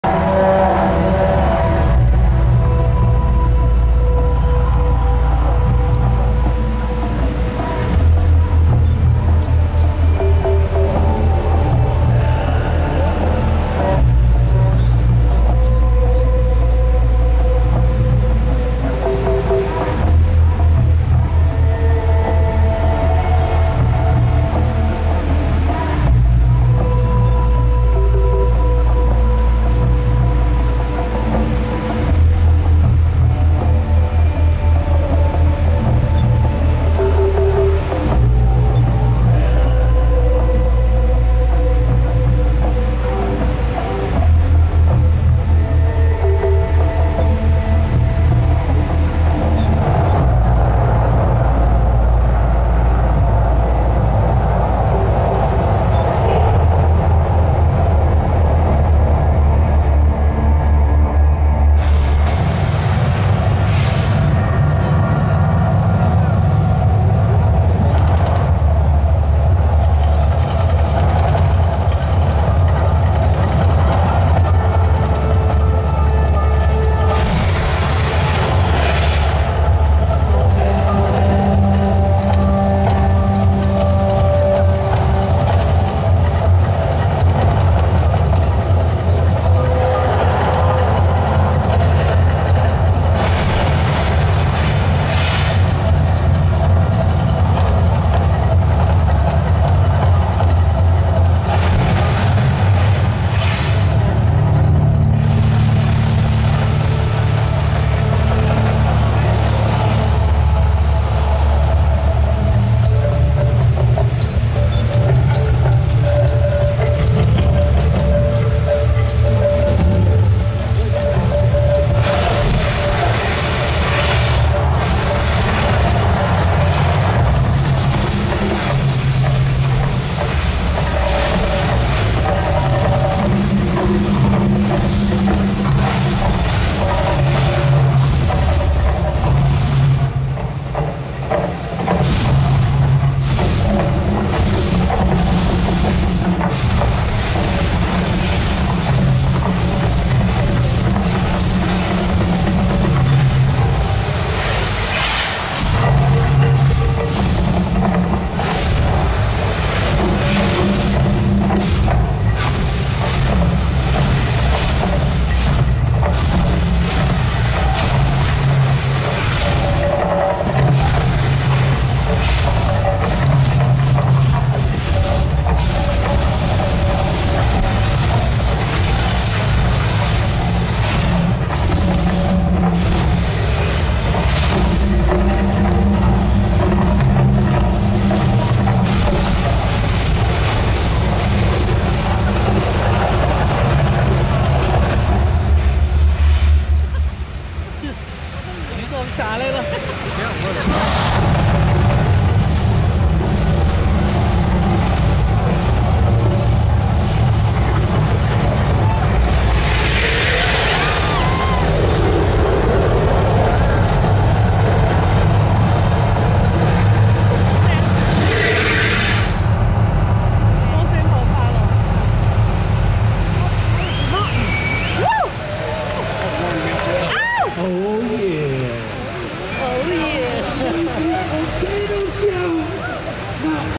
Mirage Volcano Show in Las Vegas [4K]
One of the best free shows in Las Vegas is the Volcano Eruption at the Mirage Hotel.  The volcano is located right on the Las Vegas Strip and is a really amazing show.  The shows are twice nightly on weekdays, and three times nightly Friday and Saturday.  Make sure to get there about 20 minutes early to secure a good viewing spot, it's quite a popular show.

In the winter showtimes are:
Sun-Thurs: 7pm & 8pm
Fri-Sat: 7pm & 8pm & 9pm

In the summer the show times are one hour later.